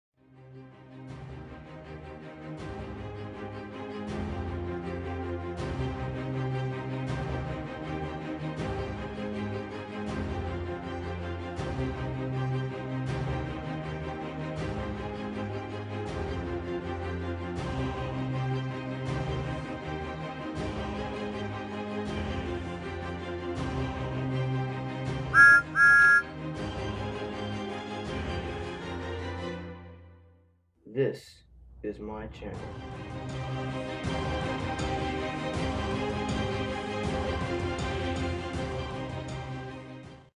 Wooden Railway Gordon trailer 2
This is a video, by me, for Wooden Railway Reviews. In one of his live streams he said that we could send him a trailer and he would advertise for us. Well, thats what I did. Music from YouTube. Video music from; Epic Blockbuster Trailer Music.